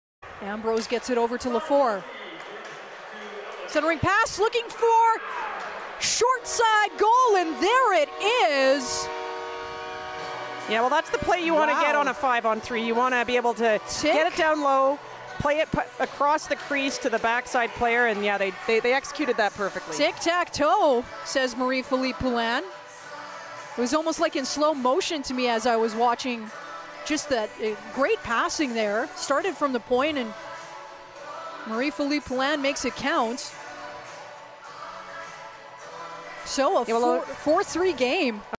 05/27 | Poulin 4-3 Goal | SDGT CAL
Marie-Philip Poulin with a beautiful backdoor goal for Team Bauer with assists from Clark and Lefort.